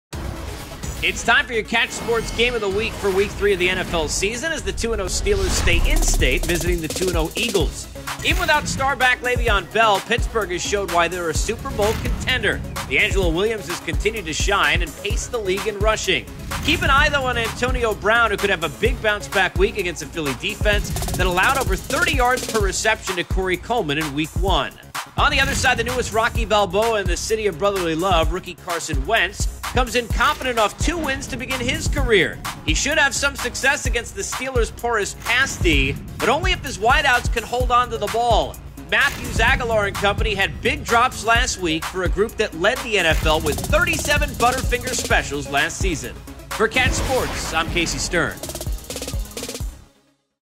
Pittsburgh Steelers at Philadelphia Eagles Preview - 9.25.16
Both teams come in 2-0 in this in-state rivalry.  Carson Wentz looks to stay perfect in the NFL while Deangelo Williams looks to fill the gap with Le'Veon Bell out.